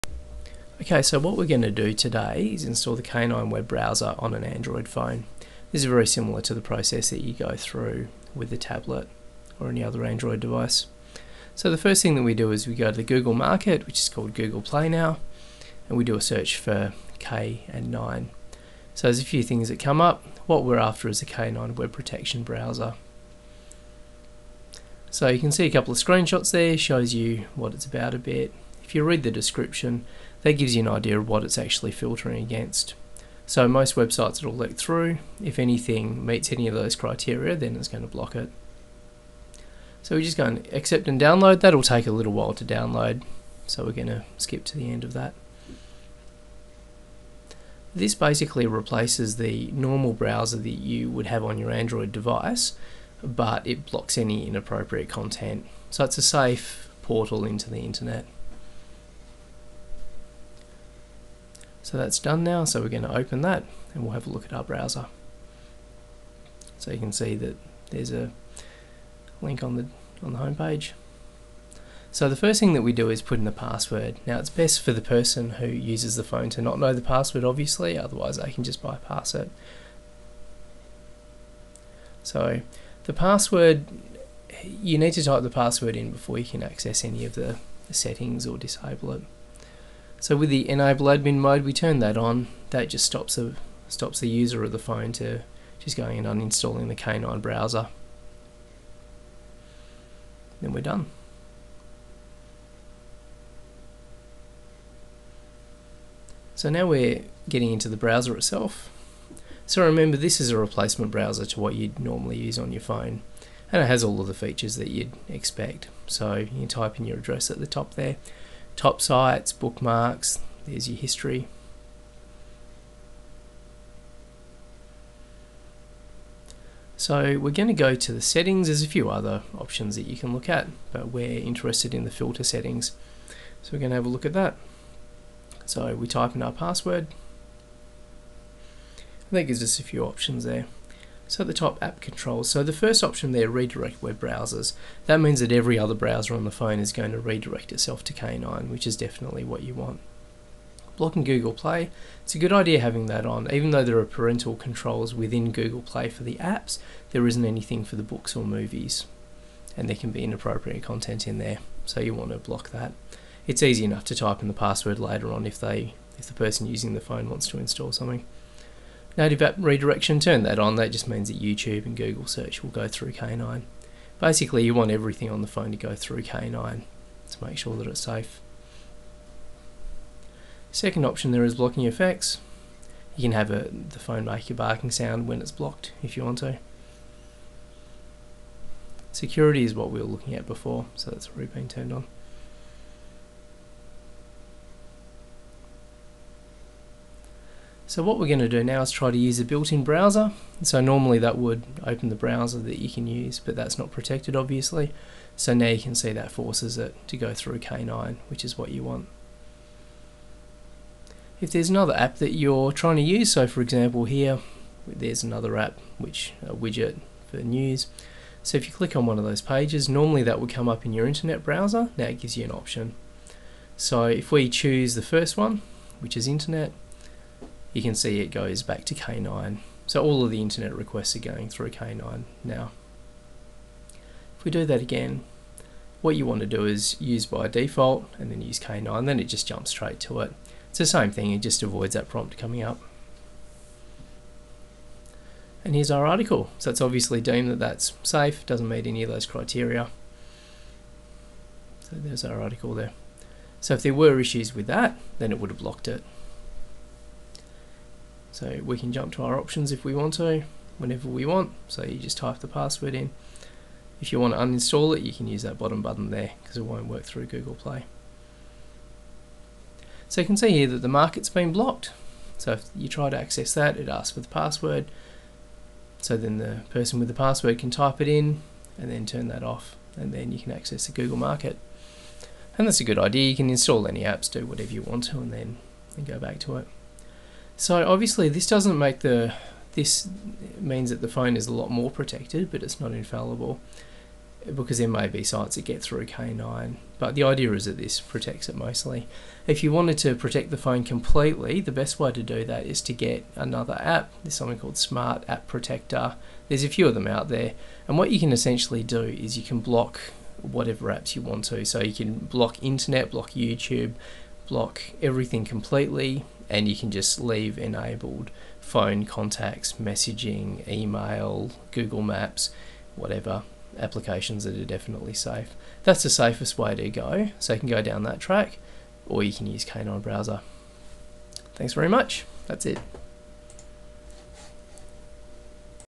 Protecting an Android phone / tablet with K9 web browser
Goes through how to install the K9 web browser on an Android phone or tablet which overrides the device's built in browser. This will protect the user from any adult content. See my other videos for protecting Windows / MacOS computers and an iPhone / iPad.